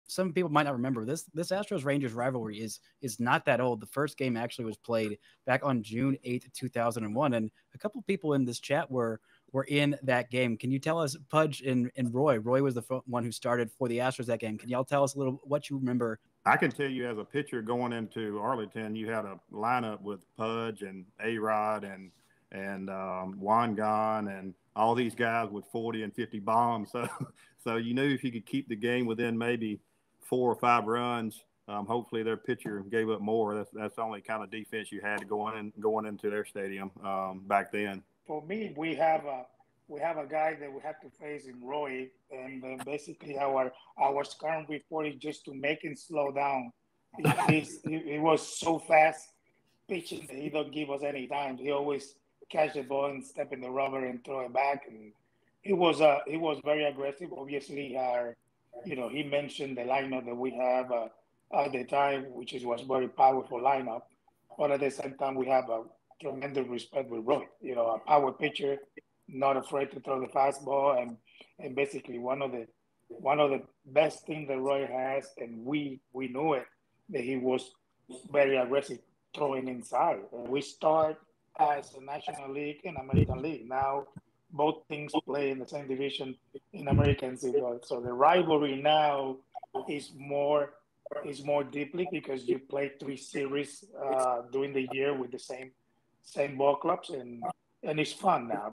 Lone Star Series 2023
Roy & Pudge on starting the lone star rivalry on HANG Lone Star Series 2023 Virtual Watch Party